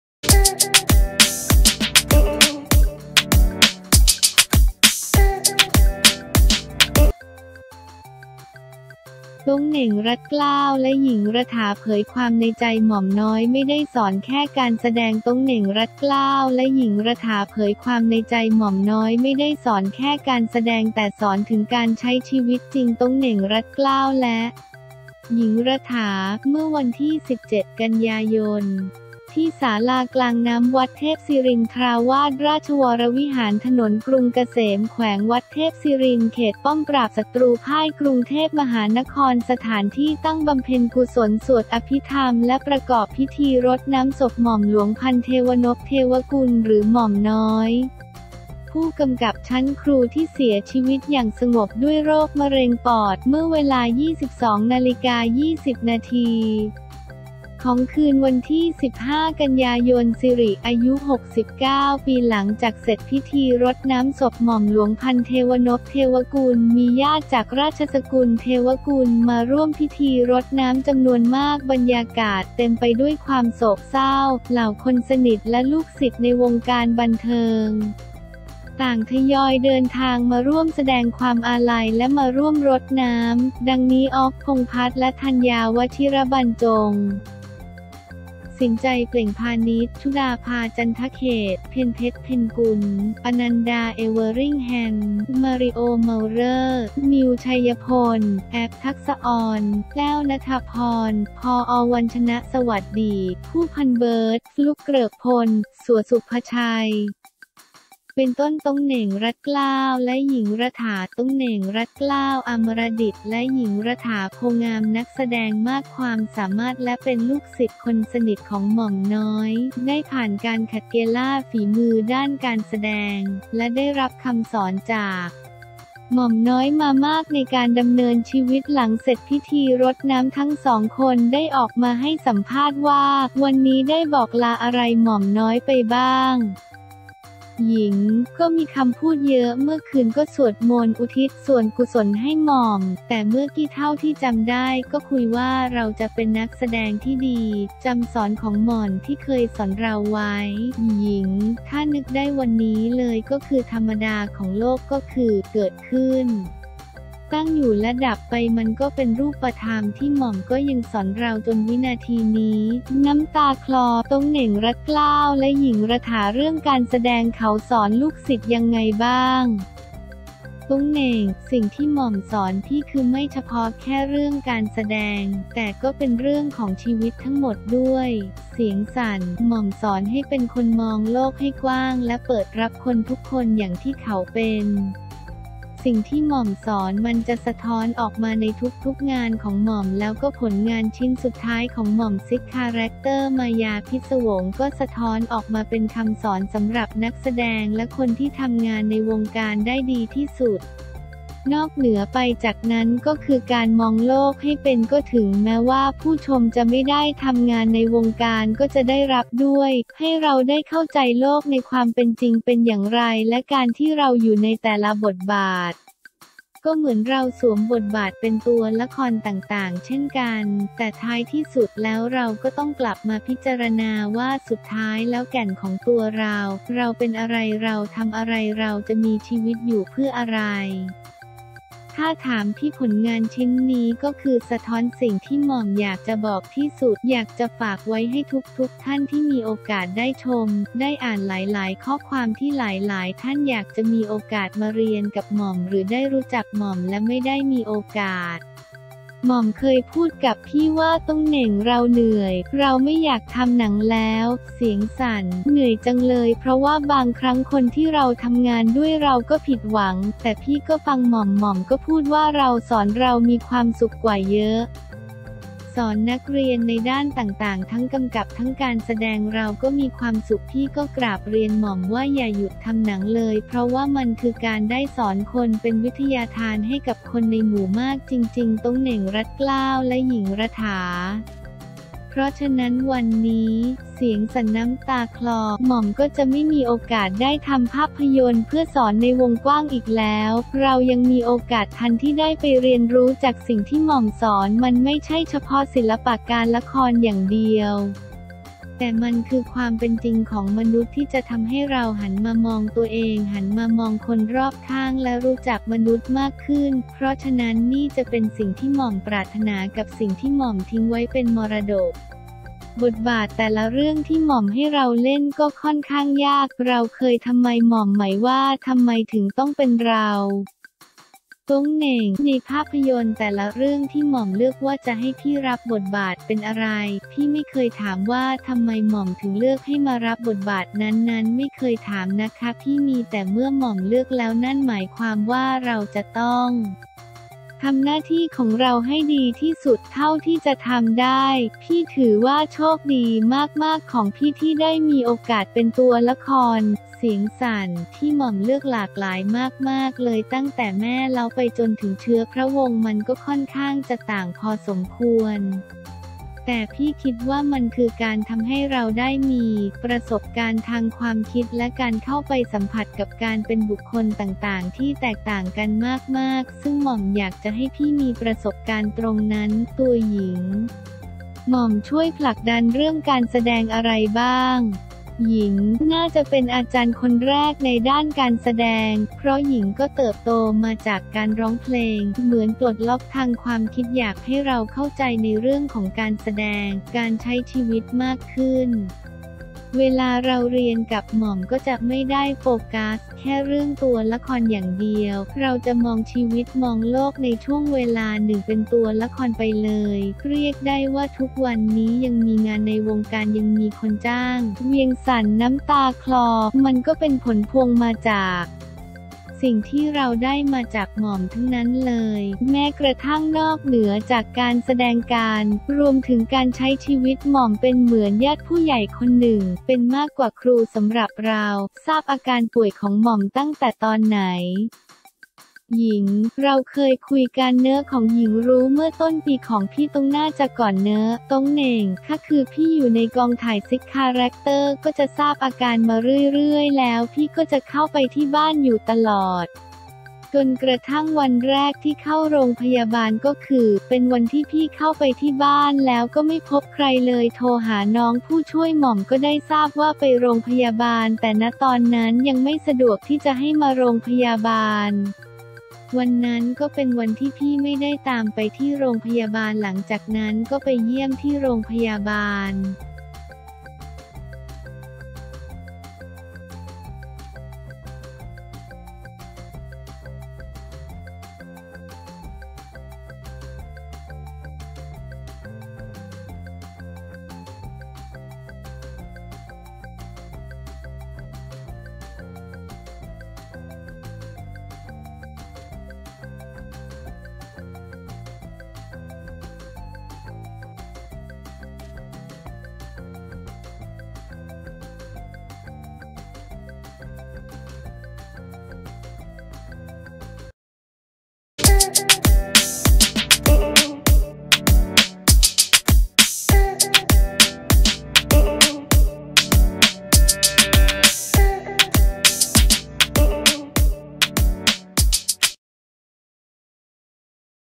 ต๊งเหน่ง รัดเกล้า และ หญิง รฐา เผยความในใจ หม่อมน้อย ไม่ได้สอนแค่การแสดง
ต๊งเหน่ง-รัดเกล้า และ หญิง-รฐา เผยความในใจ หม่อมน้อย ไม่ได้สอนแค่การแสดง แต่สอนถึงการใช้ชีวิตจริง
ต๊งเหน่ง-รัดเกล้า และ หญิง-รฐา / เมื่อวันที่ 17 ก.ย. ที่ ศาลากลางน้ำ วัดเทพศิรินทราวาส ราชวรวิหาร ถนนกรุงเกษม แขวงวัดเทพศิรินทร์ เขตป้อมปราบศัตรูพ่าย กรุงเทพมหานคร สถานที่ตั้งบำเพ็ญกุศลสวดอภิธรรมและประกอบพิธีรดน้ำศพ ม.ล.พันธุ์เทวนพ เทวกุล หรือ หม่อมน้อย ผู้กำกับฯ ชั้นครู ที่เสียชีวิตอย่างสงบด้วยโรคมะเร็งปอด เมื่อเวลา 22.20 น. ของคืนวันที่ 15 กันยายน สิริอายุ 69 ปี
หลังจากเสร็จพิธีรดน้ำศพ มล.พันธุ์เทวนพ เทวกุล มีญาติจากราชสกุลเทวกุล มาร่วมพิธีรดน้ำจำนวนมาก บรรยากาศเต็มไปด้วยความโศกเศร้า เหล่าคนสนิทและลูกศิษย์ในวงการบันเทิง ต่างทยอยเดินทางมาร่วมแสดงความอาลัยและมาร่วมรดน้ำ ดังนี้ อ๊อฟ พงษ์พัฒน์ และ ธัญญา วชิรบรรจง, สินจัย เปล่งพานิช, ชุดาภา จันทเขตต์, เพ็ญเพ็ชร เพ็ญกุล, อนันดา เอเวอร์ริ่งแฮม, มาริโอ้ เมาเร่อ, นิว ชัยพล ,แอฟ ทักษอร, แต้ว ณฐพร, พ.อ.วันชนะ สวัสดี (ผู้พันเบิร์ด) ฟลุค เกริกพล, สัว ศุภชัย, เป็นต้น
ต๊งเหน่ง-รัดเกล้า และ หญิง-รฐา
ต๊งเหน่ง-รัดเกล้า อามระดิษ และ หญิง-รฐา โพธิ์งาม นักแสดงมากความสามารถ และเป็นลูกศิษย์คนสนิทของหม่อมน้อย ได้ผ่านการขัดเกลาฝีมือด้านการแสดง และได้รับคำสอนจาก หม่อมน้อย มามากในการดำเนินชีวิต หลังเสร็จพิธีรดน้ำ ทั้งสองคนได้ออกมาให้สัมภาษณ์ว่า
วันนี้ได้บอกลาอะไรหม่อมน้อยไปบ้าง? หญิง “ก็มีคำพูดเยอะ เมื่อคืนก็สวดมนต์อุทิศส่วนกุศลให้หม่อม แต่เมื่อกี้เท่าที่จำได้ก็คุยว่าเราจะเป็นนักแสดงที่ดี” (จำสอนของหม่อนที่เคยสอนเราไว้?) หญิง “ถ้านึกได้วันนี้เลยก็คือธรรมดาของโลกก็คือ เกิดขึ้น ตั้งอยู่และดับไป มันก็เป็นรูปธรรมที่หม่อมก็ยังสอนเราจนวินาทีนี้ (น้ำตาคลอ)”
เรื่องการแสดงเขาสอนลูกศิษย์ยังไงบ้าง? ต๊งเหน่ง “สิ่งที่หม่อมสอนพี่คือไม่เฉพาะแค่เรื่องการแสดง แต่ก็เป็นเรื่องของชีวิตทั้งหมดด้วย (เสียงสั่น) หม่อมสอนให้เป็นคนมองโลกให้กว้างและเปิดรับคนทุกคนอย่างที่เขาเป็น สิ่งที่หม่อมสอนมันจะสะท้อนออกมาในทุกๆ งานของหม่อม แล้วก็ผลงานชิ้นสุดท้ายของหม่อม Six Characters มายาพิศวง ก็สะท้อนออกมาเป็นคำสอนสำหรับนักแสดงและคนที่ทำงานในวงการได้ดีที่สุด